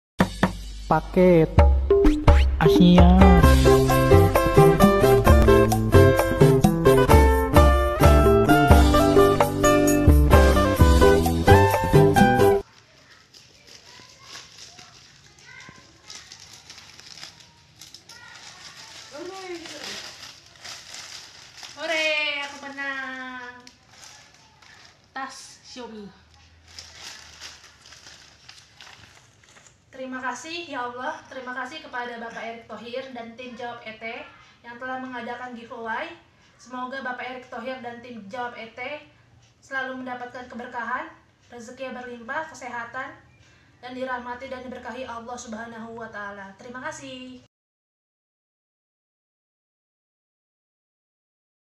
Ransel Mi Keren Dari Tim JawabET Teruntuk Kak Mega Di Tangerang, Banten.
Selamat buat akun IG @mega_1991 Dari Tangerang Banten

Kuis
Periode:  1-7 November 2021

Buruan ikutan karena kita akan bagi-bagi giveaway setiap hari, siapa tahu kamu beruntung.